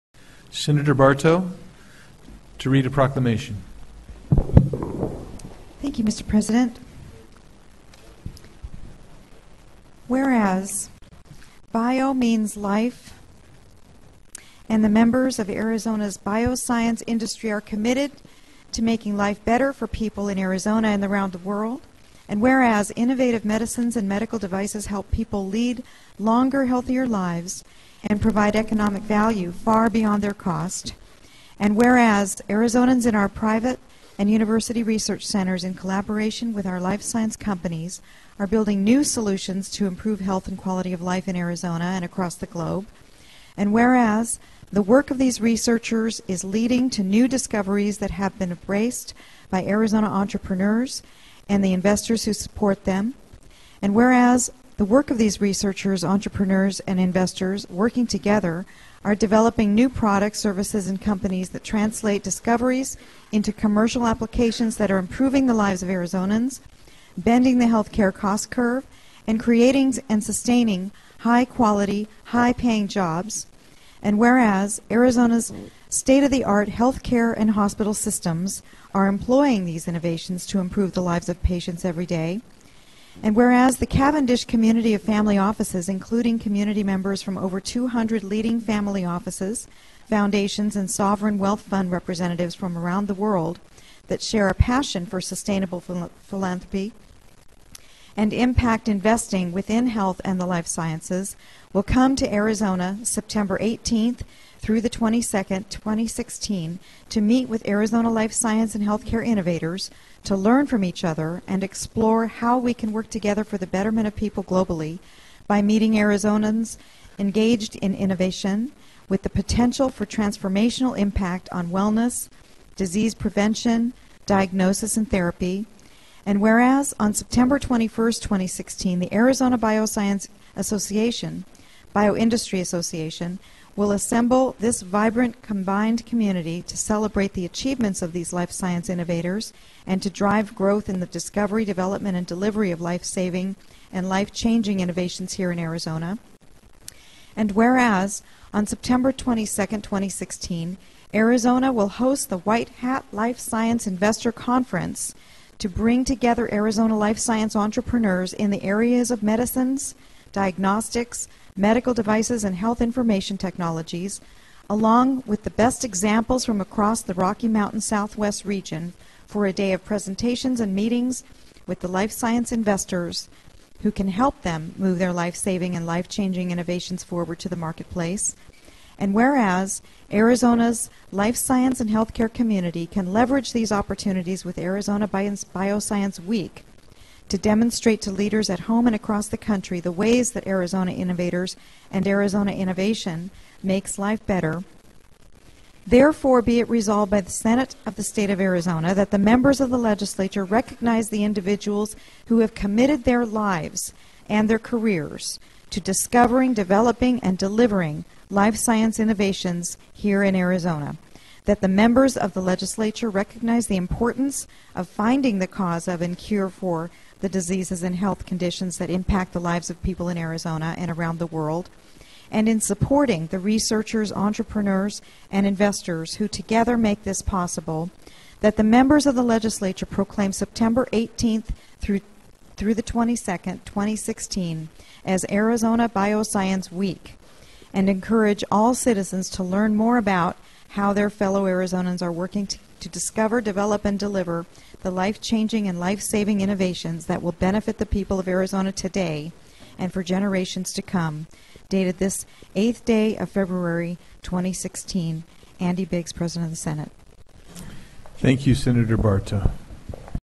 AZBW Senate Proclamation 2016
Arizona Senator Nancy Barto, Chairman of the Health and Human Services Committee reads the Senate Proclamation recognizing the work of Arizona's Bioindustry and proclaiming the week of September 18, 2016  as Arizona Bioscience Week.